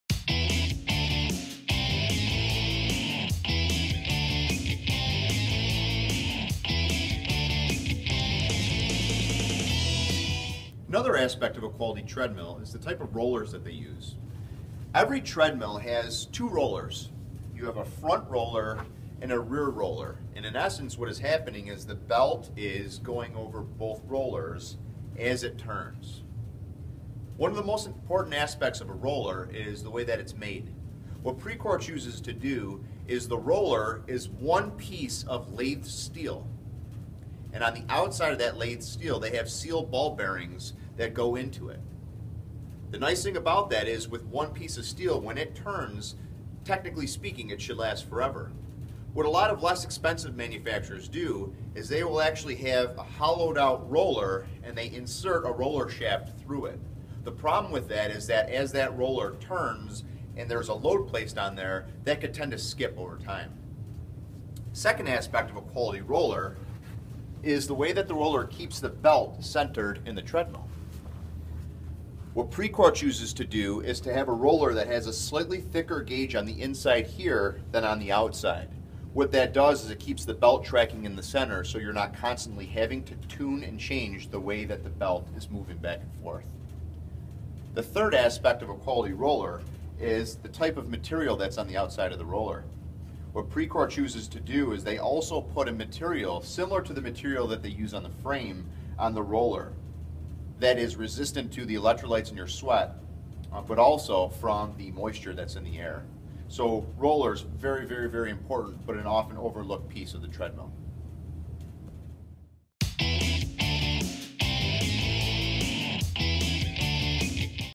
Components of a Treadmill - Rollers
The Rollers are one of the most important components of a Treadmill.  What makes one Roller better quality then another? Let the G&G experts show you why good Rollers are so important for Treadmill.